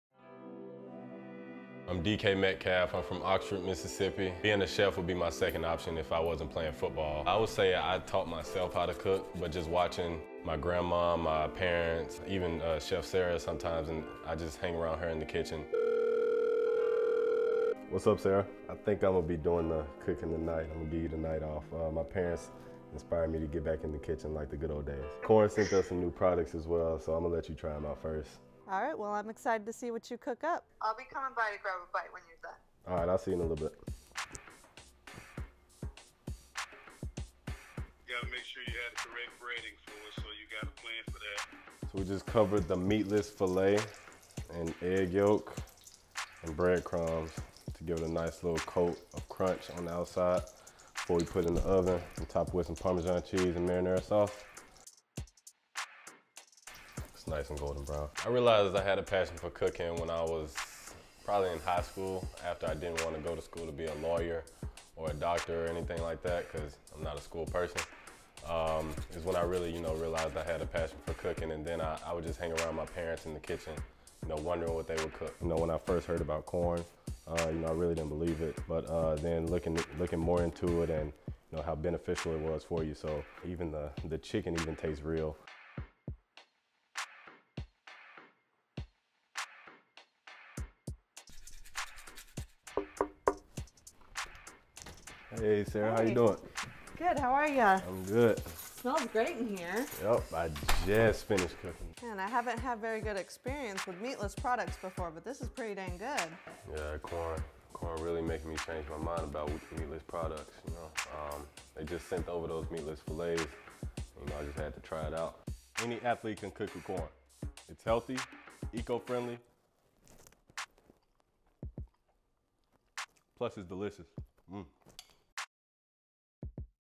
Quorn Partner – DK Metcalf
See how wide receiver and Quorn partner, DK Metcalf is FueledByQuorn and how any athlete can cook with Quorn meatless protein.